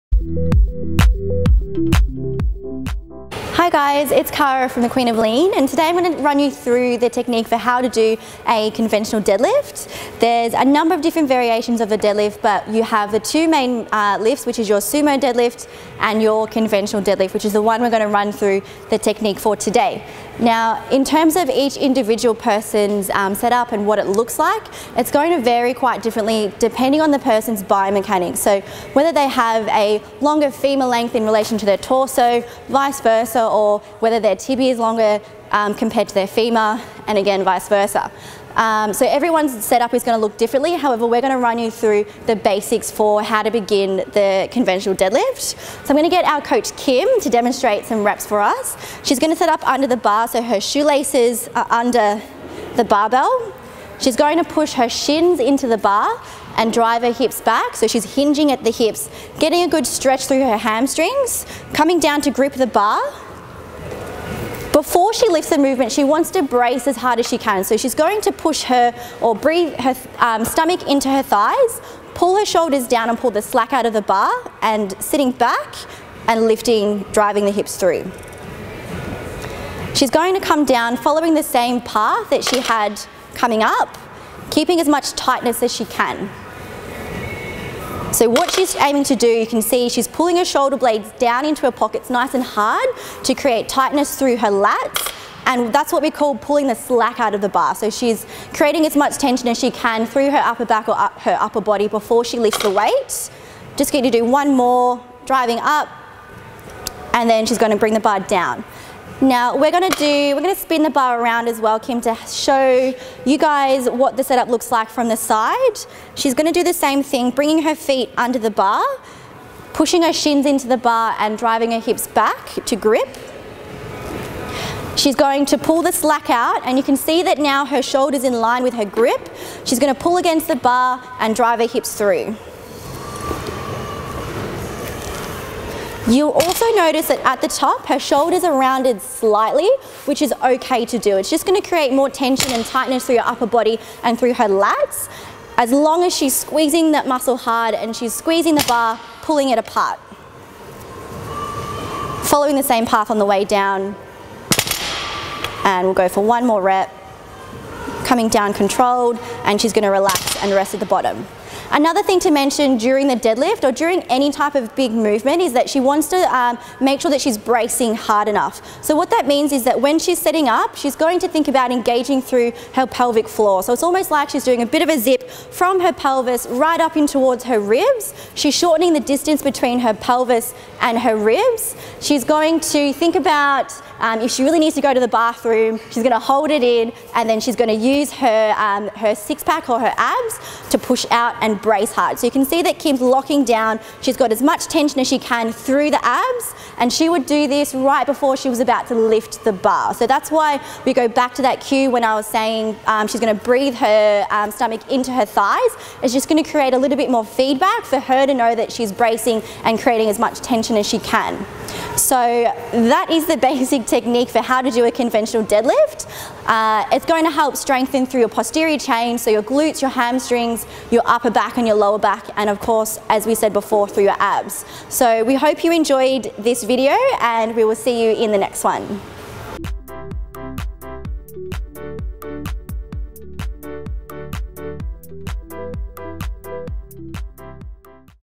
Conventional Deadlift - Technique Clip
Watch this clip to discover how to effectively do a deadlift. This is an awesome exercise to strengthen legs, glutes and the whole Posterior chain!